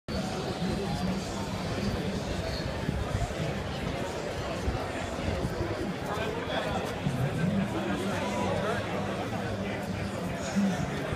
DJI Booth CineGear 2016
A quick view of the lot street in front of DJI's boot at the CineGear Expo at Paramount Studios Lot.